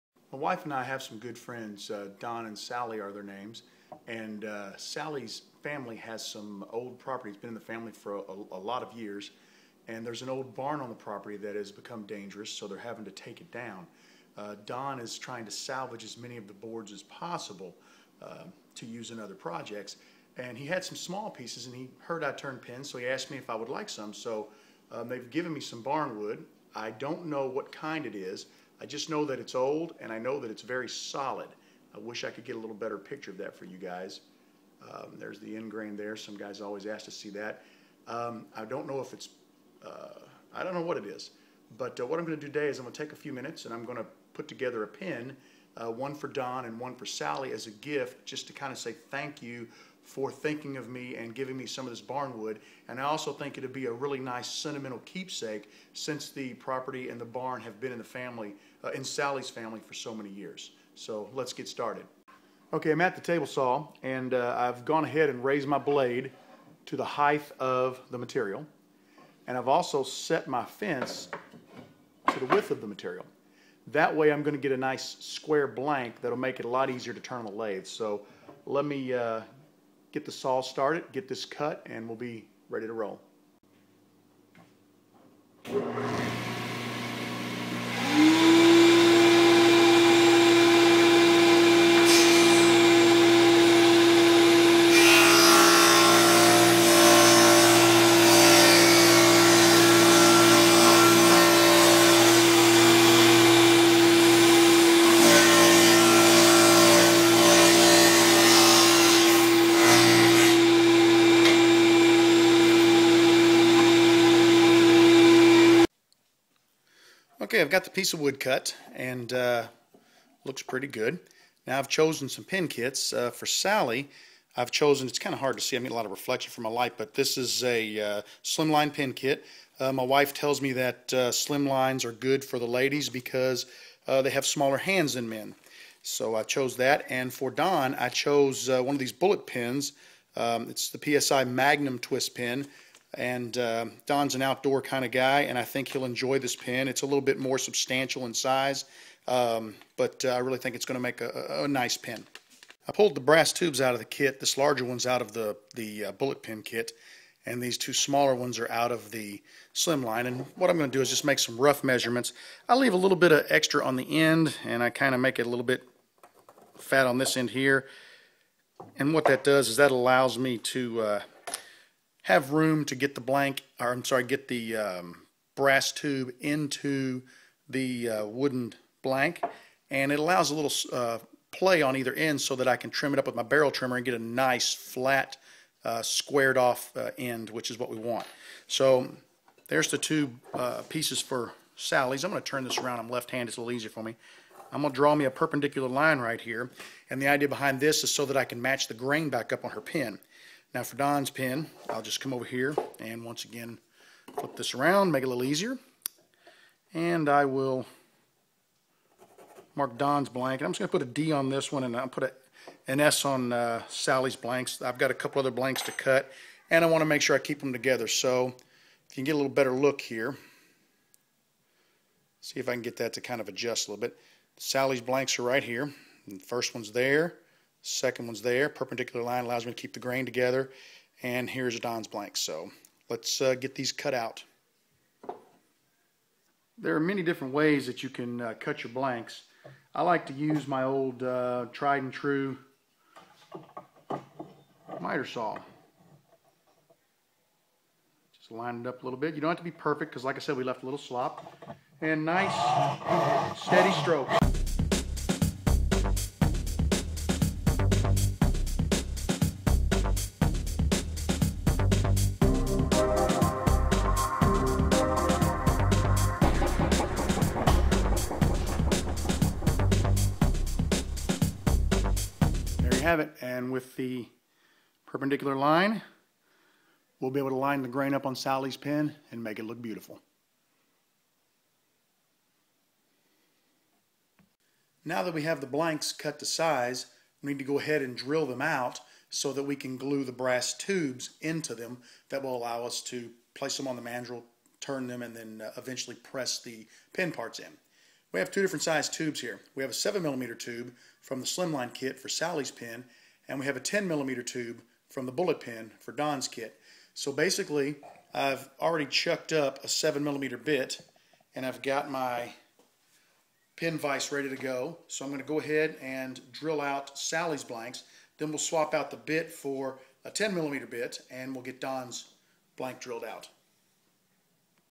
Barn Wood Pens Part 1 of 7
Barn Wood Ink Pens

In this video series, I walk through the process of making two ink pens, a Slim Line and a Bullet pen, from some barn wood that was given to me by some good friends. That barn was made of White Oak and well over a 150 years old. It had become dangerous and needed to be dismantled. The owner was repurposing the larger boards and offered me some of the smaller stock. I made these pens as gifts and keep sakes for the family.

In Video 1, I cover the following topics:

Intro to the project.
Introduce the wood being used.
Size the wood.
Select the pen kits.
Size the pen blanks.
Cut the pen blanks.
Discuss the brass tube sizes.